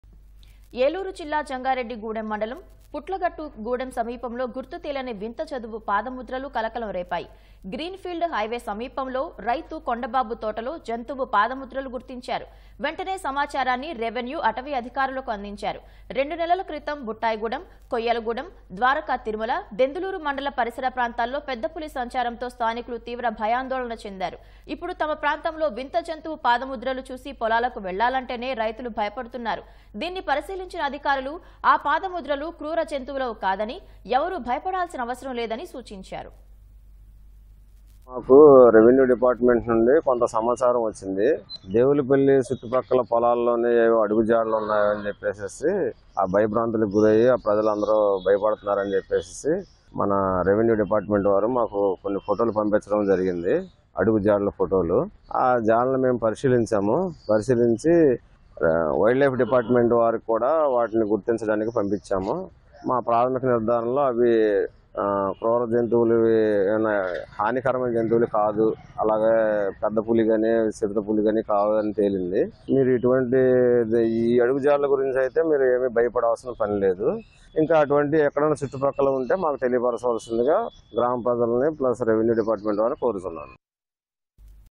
పుట్లగట్టుగూడెంలో గుర్తుతెలియని వింత జంతువు పాదముద్రల కలకలం | Strange Animal Footprints Hulchul
ఏలూరు జిల్లా జంగారెడ్డిగూడెం మండలం పుట్లగట్టుగూడెం సమీపంలో గుర్తుతెలియని వింత జంతువు పాదముద్రలు కలకలం రేపాయి. గ్రీన్ ఫీల్డ్ హైవే సమీపంలో రైతు కొండబాబు తోటలో జంతువు పాదముద్రలు గుర్తించారు. వెంటనే సమాచారాన్ని రెవెన్యూ, అటవీ అధికారులకు అందించారు.
2 నెలల క్రితం బుట్టాయిగూడెం, కొయ్యలగూడెం, ద్వారకాతిరుమల, దెందులూరు మండల పరిసర ప్రాంతాల్లో పెద్దపులి సంచారంతో స్థానికులు తీవ్ర భయాందోళన చెందారు. ఇప్పుడు తమ ప్రాంతంలో వింత జంతువు పాదముద్రలు చూసి పొలాలకు వెళ్లాలంటేనే రైతులు భయపడుతున్నారు. దీన్ని పరిశీలించిన అధికారులు. ఆ పాదముద్రలు క్రూర జంతువులవి కాదని... ఎవరూ భయపడాల్సిన అవసరం లేదని సూచించారు.